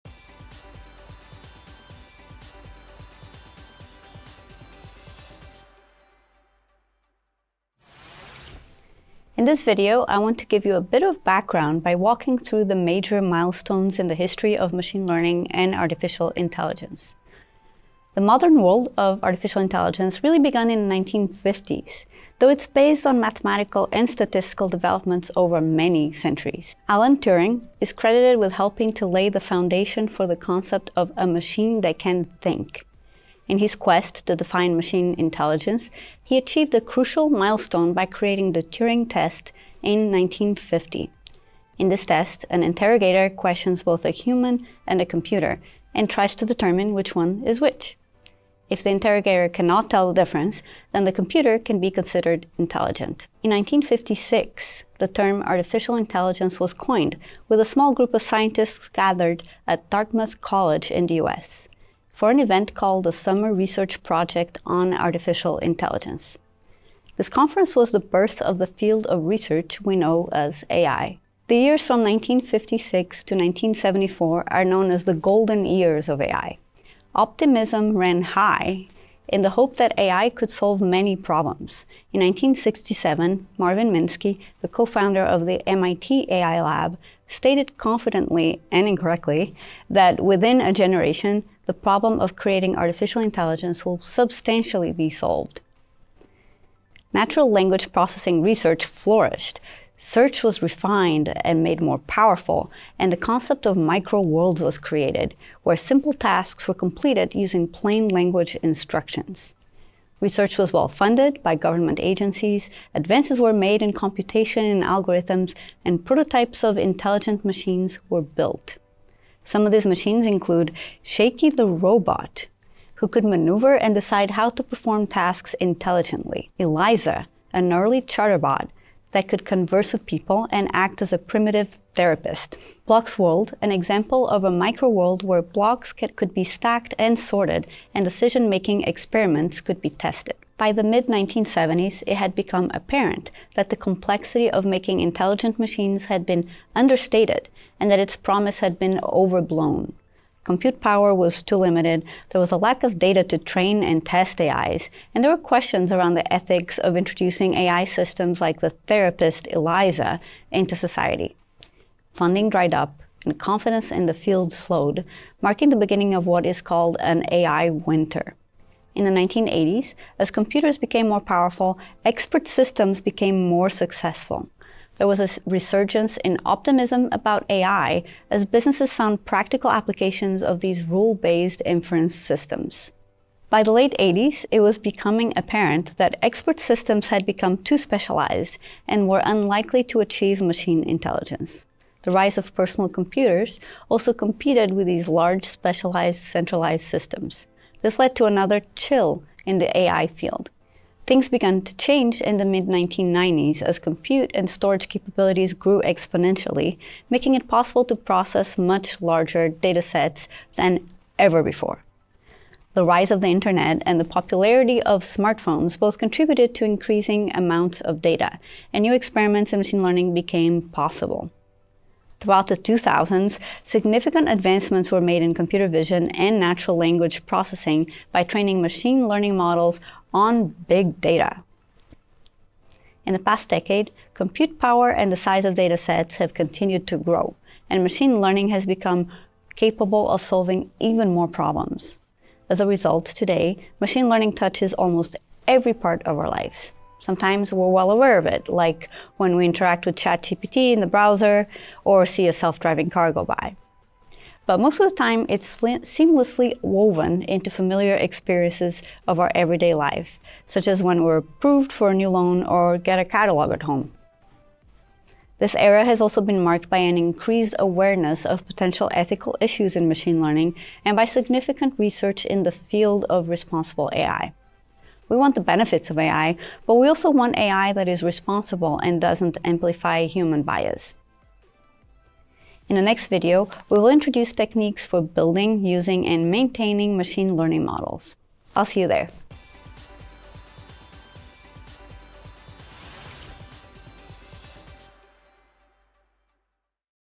The history of Machine Learning [Part 2] | Machine Learning for Beginners
📜🤖🧠 Dive into the fascinating history of machine learning and artificial intelligence in Lesson 2 of our Machine Learning for Beginners course, presented by Bea Stollnitz, a Principal Cloud Advocate at Microsoft. Discover the milestones and challenges that led to the development of the powerful AI technologies we have today.

In this video, we'll explore:
✅ The Turing Test and its impact on AI
✅ The birth of AI and the golden years
✅ AI winter and the rise of expert systems
✅ The emergence of big data and its influence on machine learning

Understanding the history of machine learning provides valuable context for the current state of the field and helps us appreciate the advances that brought us to where we are now. Don't miss out on this exciting journey through the evolution of AI and ML! 🌟

Make sure to subscribe and hit the notification bell 🔔 so you don't miss the next video in this series, where we'll introduce techniques for building, using, and maintaining machine learning models. See you there!

0:00 - Intro
0:18 - Alan Turing and the Turing test
0:53 - The Dartmouth Summer Research Project on AI
1:12 - The golden years of AI
2:20 - The AI winter
2:50 - Resurgence and fall of AI for expert systems
3:30 - Growth in AI driven by more data and more powerful hardware
4:25 - Increased awareness of ethical and responsible AI

This course is based on the free, open source, 26 lesson ML For Beginners curriculum from Microsoft, which can be found